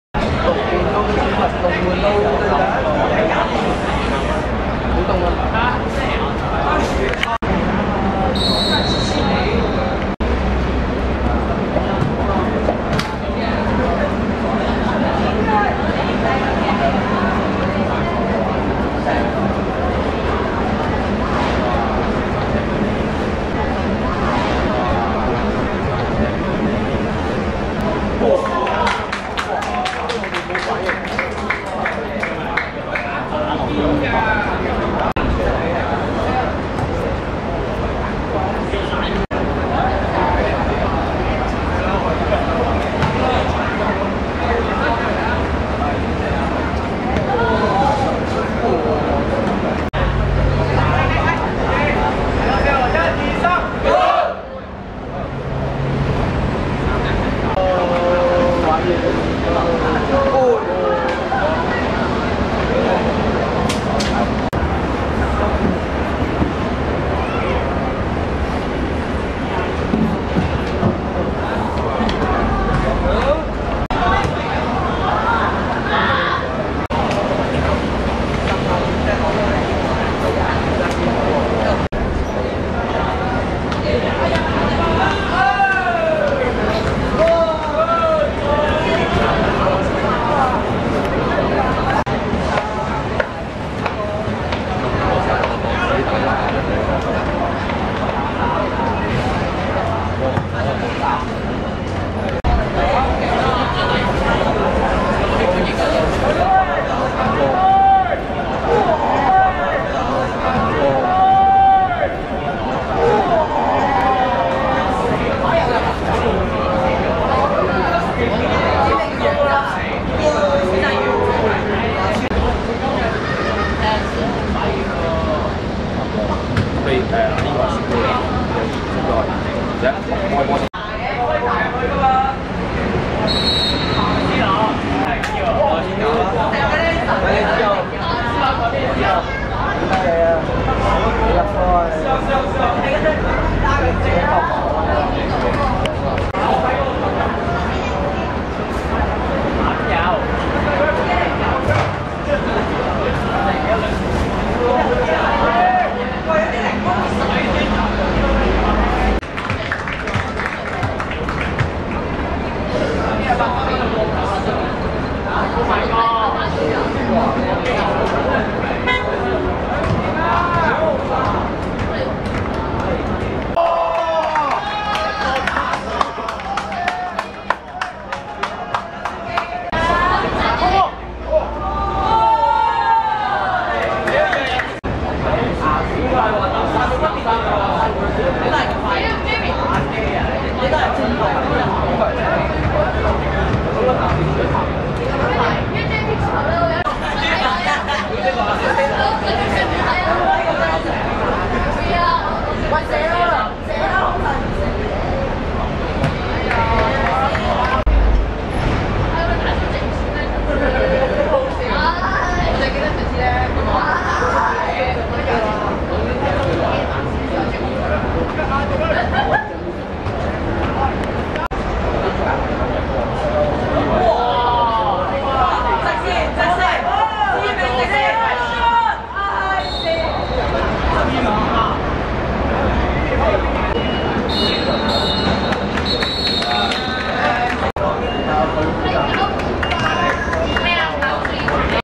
仁濟二中vs邱子田(2016.3.5.屯門學界足球乙組)精華
2016.3.5.兆麟運動塲 10:30 仁濟二中(白) 1 : 3 邱子田(粉紅)